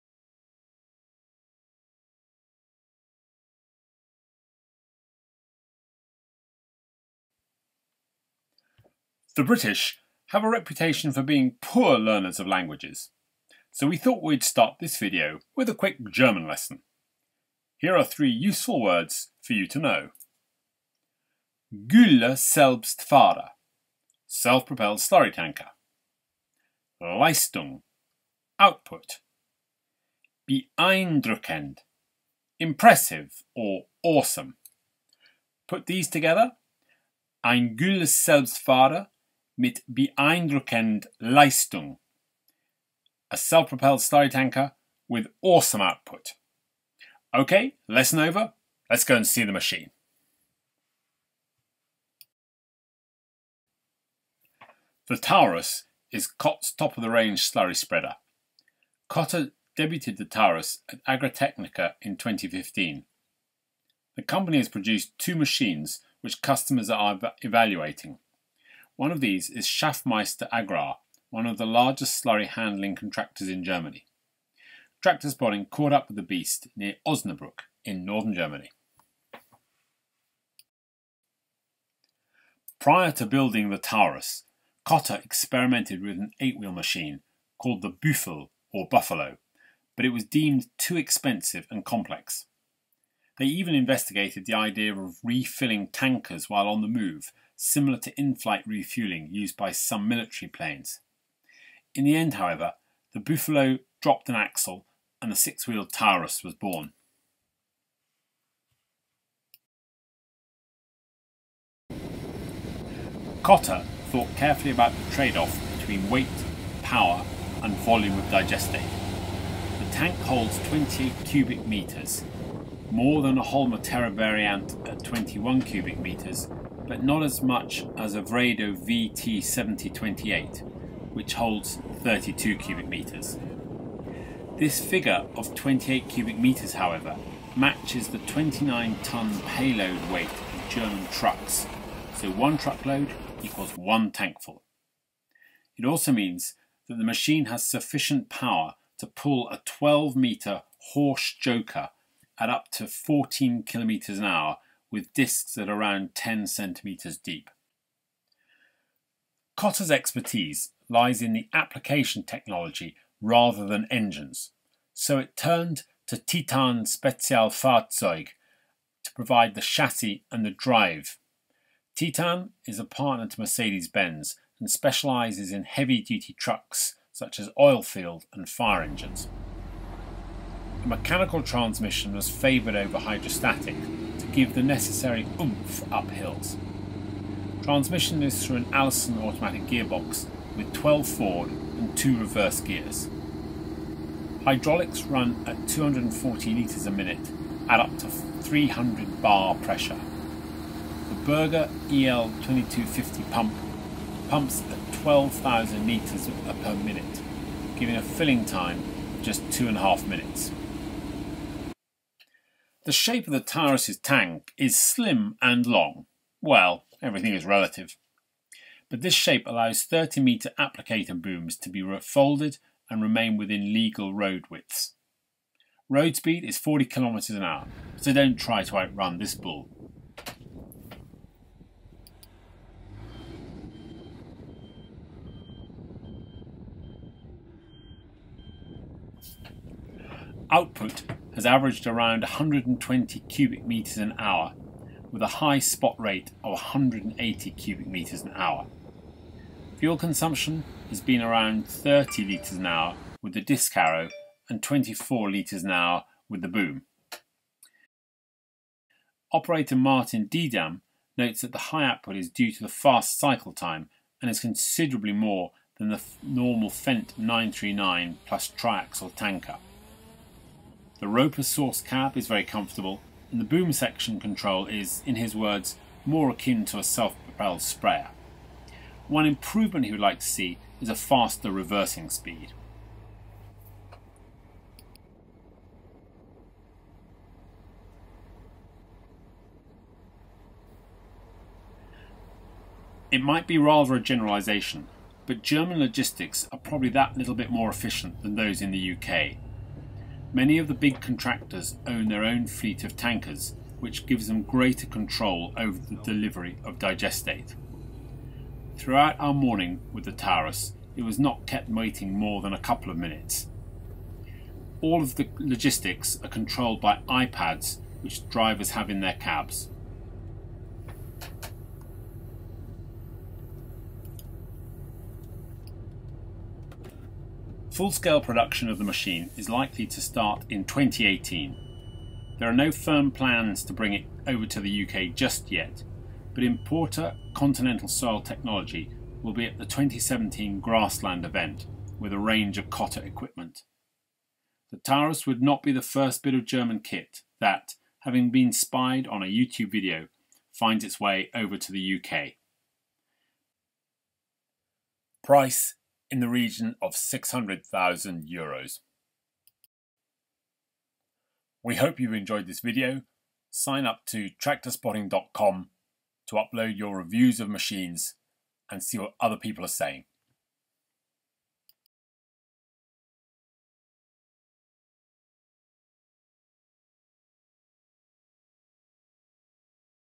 Taurus 2803
Taurus Self Propelled Slurry Tanker working near Osnabruck
Gülle Selbstfahrer
Garant Kotte